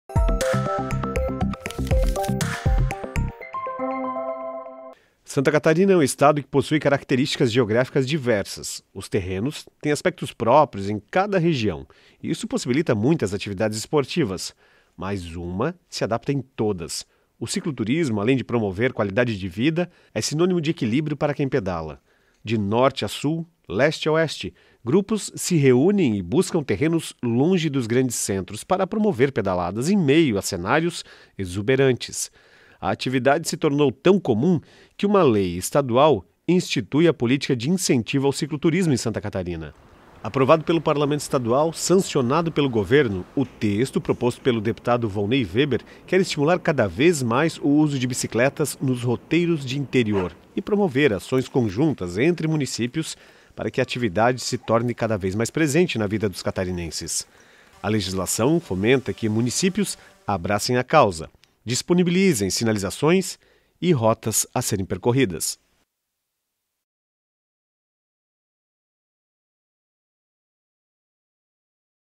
É Notícia - Lei Catarinense vai estimular o Cicloturismo - 13/06/22
Uma Lei de origem parlamentar quer estimular políticas de incentivo ao cicloturismo. A intenção é promover o uso de bicicletas em roteiros pelo interior. Com a Legislação, os amantes das bicicletas poderão a longo prazo ter mecanismos de suporte e identificação de rotas.